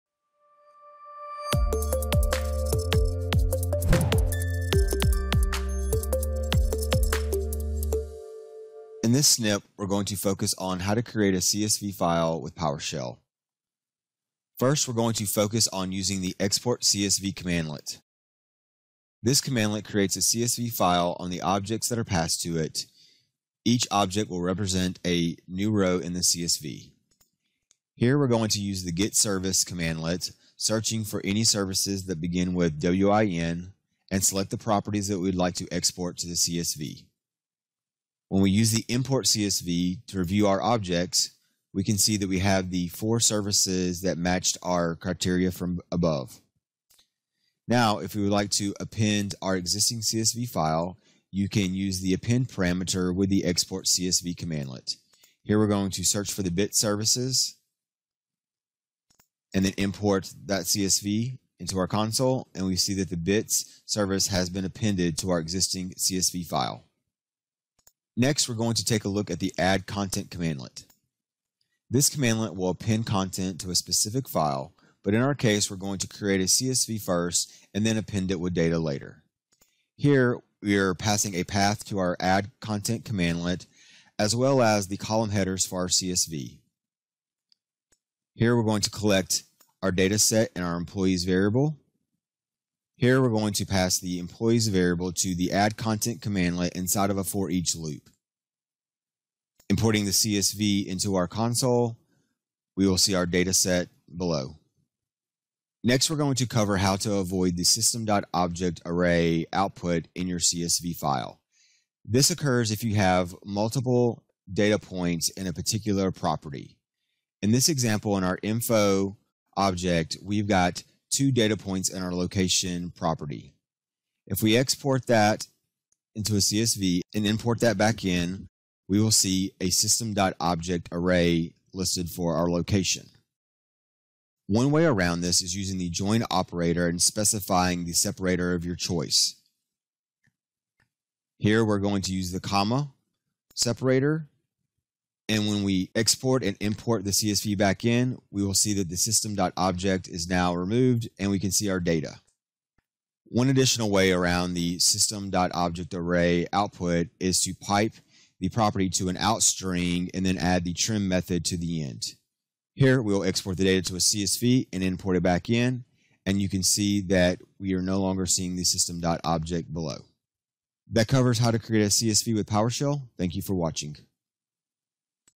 How To Create CSV Files With PowerShell
If you work with PowerShell, eventually you'll work with CSV's.  Sometimes, you'll need to be able to create the CSV from various PowerShell objects.  In this video, Jason shows us how awesome he is yet again and goes into how to create CSV files with PowerShell using his Export-Csv cmdlet juju.